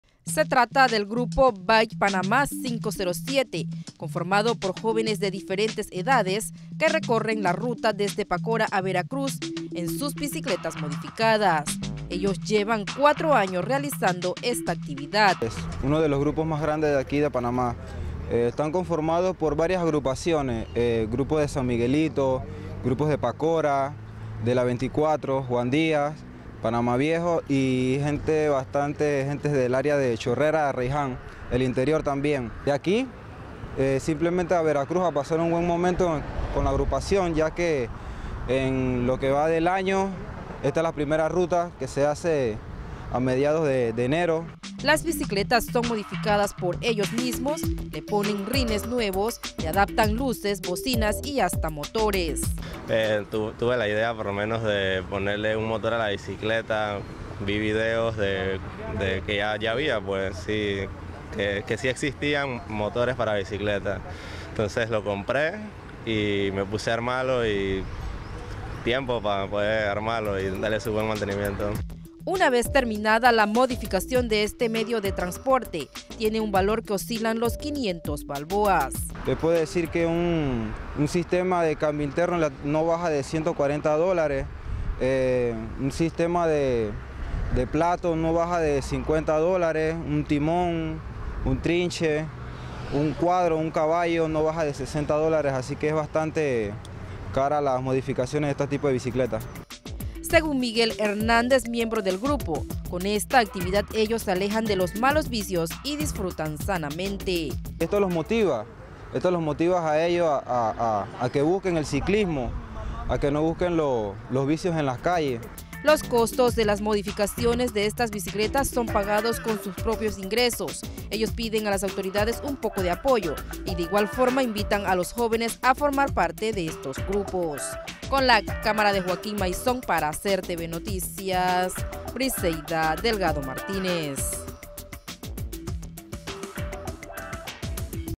ACTIVIDADES DE VERANO MODIFICACION DE BICICLETAS
En Panamá, existen diversos grupos de ciclistas entre ellos estos que aparte de montar como lo hacen todos los demás,  también modifican sus maquinas con piezas de alta gama y muchas también fabricadas en casa.